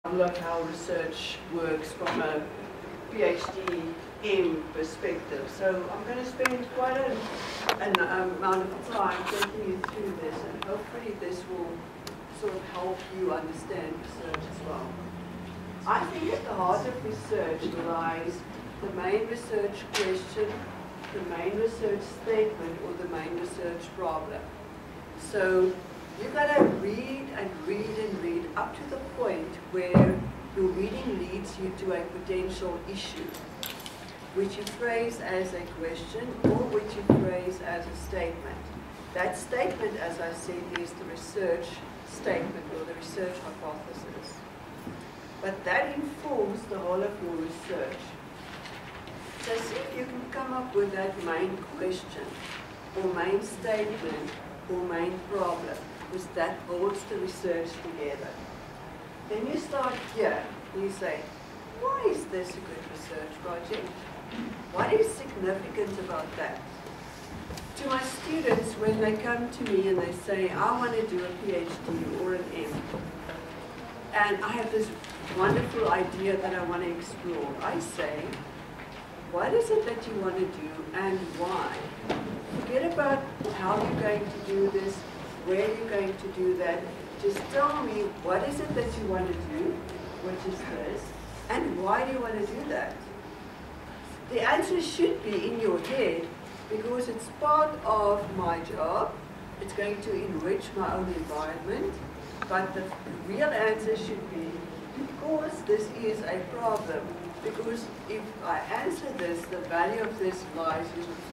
RESEARCH DESIGN (Clip 1): Main research question/statement
The main research question or statement/hypothesis informs the whole of one's doctoral research. You should consider what makes your project a good research project, and what is significant about it, and the fundamental question to consider is what is it that you want to research and why?

This video is from a Research Design workshop given by Prof Rene Pellissier at the Postgraduate School at the University of the Free State South Africa, September 2013.